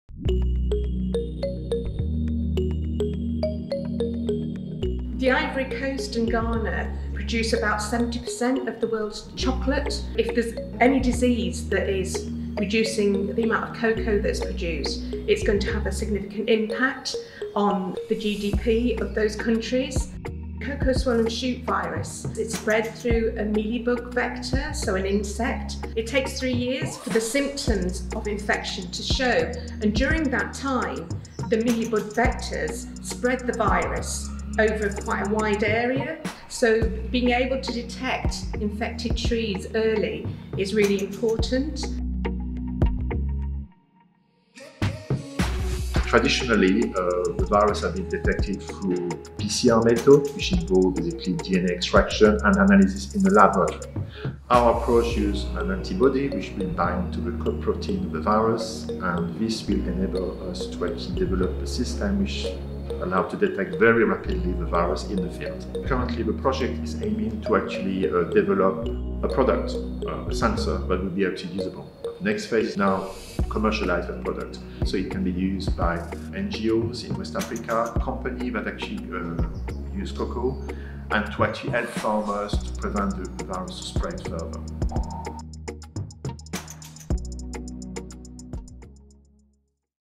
UWE Bristol scientists devised a biosensor to tackle cocoa disease
A new biosensor developed by UWE Bristol scientists will help tackle the cacao swollen shoot virus (CSSV) and protect West Africa’s endangered cocoa industry.